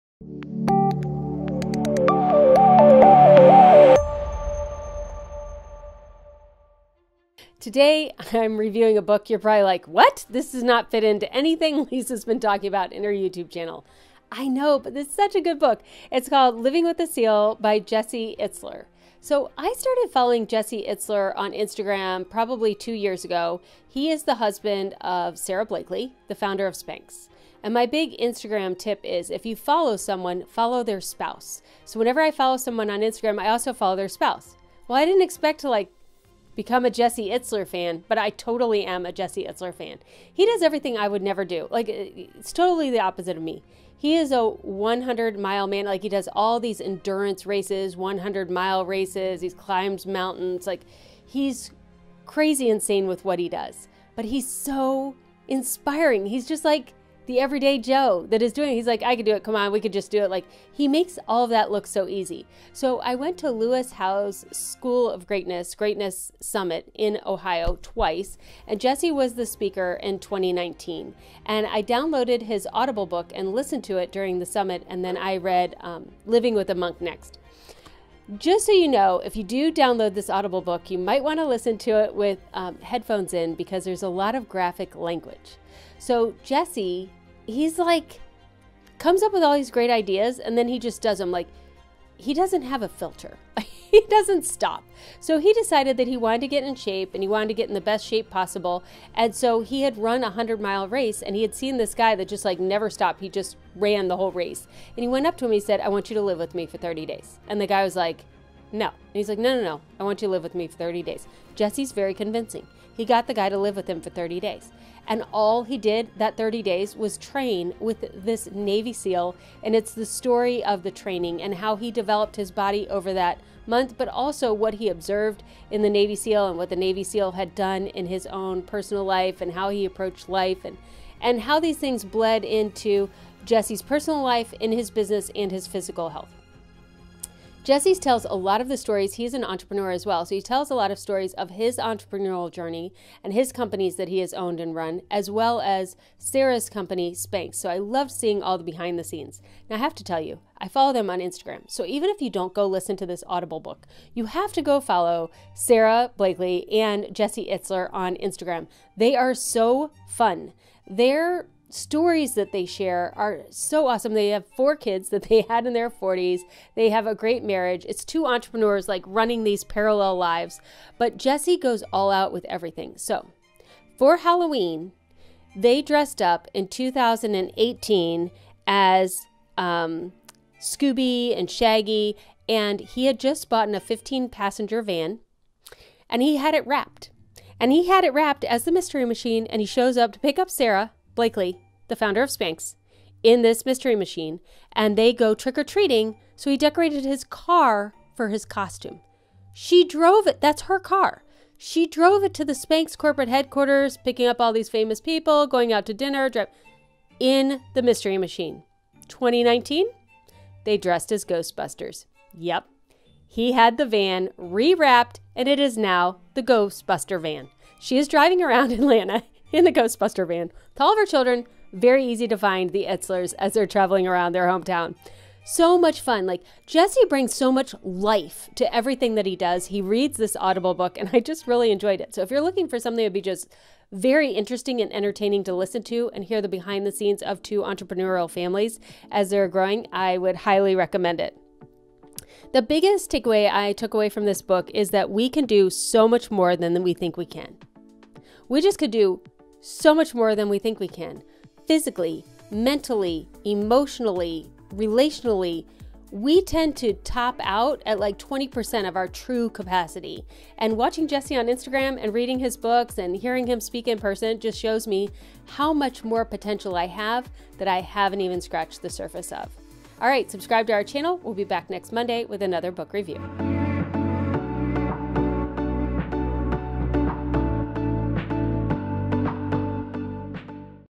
Jesse Itzler | Living With a SEAL | Book Review by Lisa Woodruff of Organize 365
This week, I review the book Living with a SEAL: 31 Days Training with the Toughest Man on the Planet by Jessie Itzler.

I first learned about Jessie Itzler by following him on Instagram. He is an everyday Joe doing all kinds of inspiring challenges. Jessie decided he wanted to get in the best shape possible, so he had run a 100 mile race. He talked a Navy SEAL into living with him and training him for 30 days.

Jessie brings so much life to everything he does, and I found this book highly entertaining. This book shows us how we can do so much more than we think we can.

(Note - the audiobook has some graphic language - consider headphones)